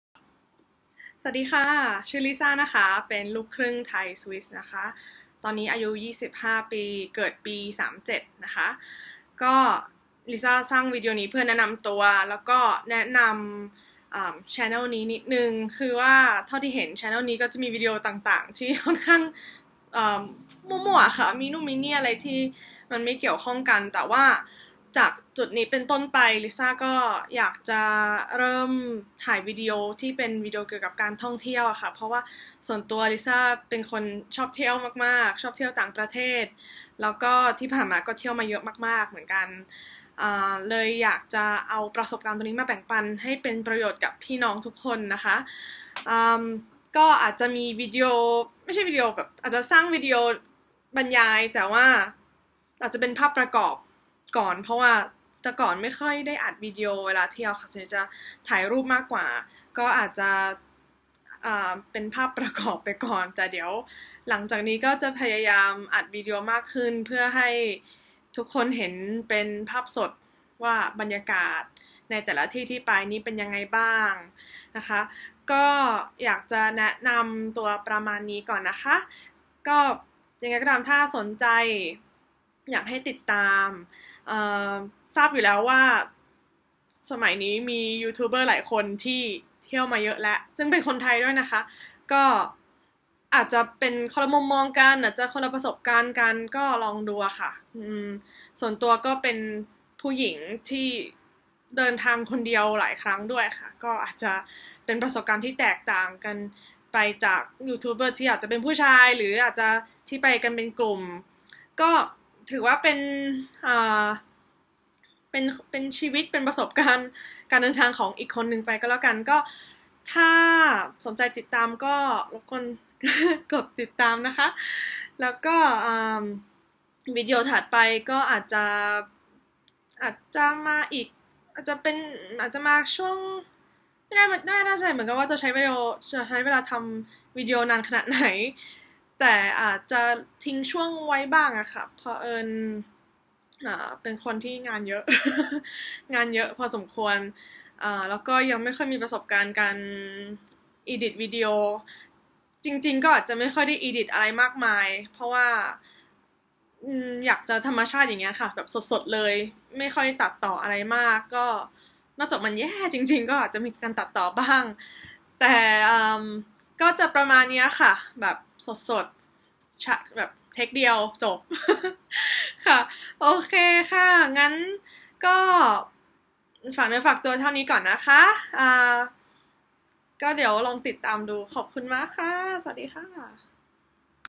ลิซ่าแนะนำตัวเป็นทางการจ้าา ^_^
แนะนำตัวและเกริ่นๆเกี่ยวกับวีดีโอท่องเที่ยวที่คนรอบข้าง (หรือใครก็ได้) อยากติดตามค่ะ หวังว่าจะเป็นประโยชน์ให้กับทุกคนนะคะ ขอบคุณค่ะ ^_^

สนับสนุนการเดินทางของลิซ่าได้ผ่าน